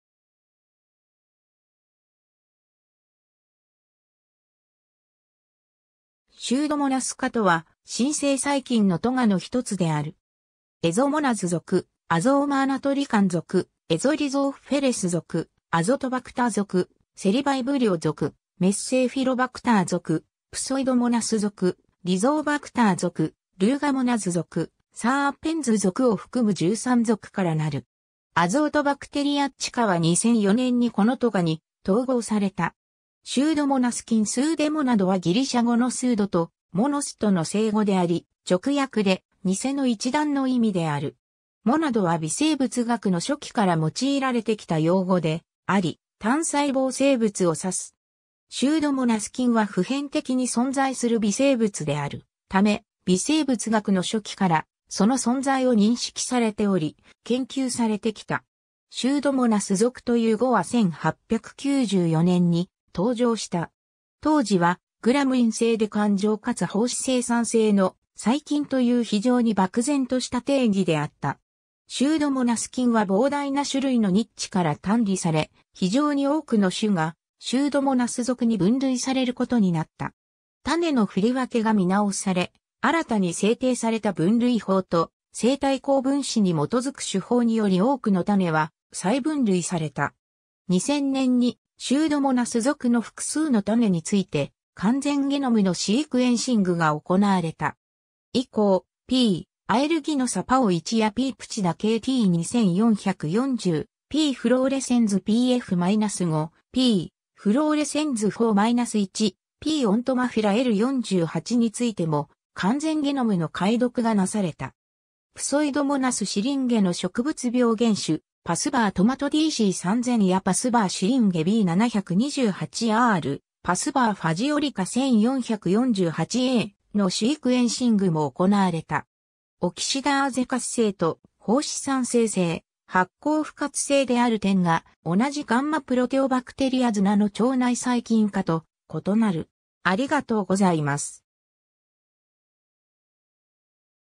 シュードモナス科
シュードモナス科（Pseudomonadaceae）とは真性細菌の科の一つである。
Azomonas属、Azomonotrichon属、Azorhizophilus属、Azotobacter属、Cellvibrio属、Mesophilobacter属、Pseudomonas属（タイプ属）、Rhizobacter属、Rugamonas属、Serpens属を含む13属から成る。
Azotobacteriaceae科は2004年にこの科に統合された。
 シュードモナス菌Pseudomonadはギリシャ語のpseudo (ψευδο - 偽の)とmonos (μονος - 一団)との成語であり、直訳で「偽の一団」の意味である。
"monad"は微生物学の初期から用いられてきた用語であり、単細胞生物を指す。
シュードモナス菌は普遍的に存在する微生物であるため、微生物学の初期からその存在を認識されており、研究されてきた。
シュードモナス属という語は1894年に登場した。
当時は、グラム陰性で桿状かつ胞子生産性の細菌という非常に漠然とした定義であった。
シュードモナス菌は膨大な種類のニッチから単離され、非常に多くの種がシュードモナス属に分類されることになった。
種の振り分けが見直され、新たに制定された分類法と、生体高分子（核酸やタンパク質）に基づく手法により多くの種は再分類された。
 2000年にシュードモナス属の複数の種について完全ゲノムのシークエンシングが行われた。
以降、P. aeruginosa PAO1 (2000)やP. putida KT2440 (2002)、P. fluorescens Pf-5 (2005)、P. fluorescens PfO-1、P. entomophila L48についても完全ゲノムの解読がなされた。
Pseudomonas syringaeの植物病原種 ― pathovar tomato DC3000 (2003)やpathovar syringae B728a (2005)、pathovar phaseolica 1448A (2005) ― のシークエンシングも行われた。
 オキシダーゼ活性と胞子産生性、発酵不活性である点が、同じガンマプロテオバクテリア綱の腸内細菌科と異なる。